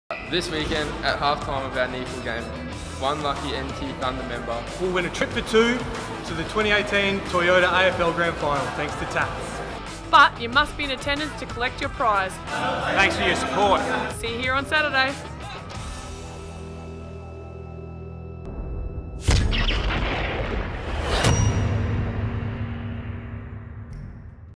2018 Tatts Members' Half-Time Draw
This Saturday at half time of our Southport Sharks Round 20 NEAFL clash at TIO Stadium, one lucky NT Thunder member will be walking away with the ultimate 2018 Toyota AFL Grand Final experience.  The winning member must be present at the ground to claim the prize which includes:
- $500 spending money
- 2x AFL Toyota Grand Final tickets
- 2x return flights to Melbourne
- 2 night's accommodation